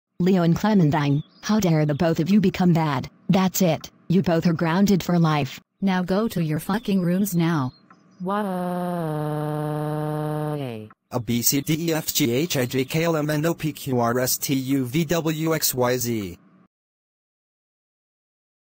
Leo and Clementine Gets Grounded For The First Time [READ DESCRIPTION]
They’re Caillou’s friends from his school. And btw, this is the series I’m doing.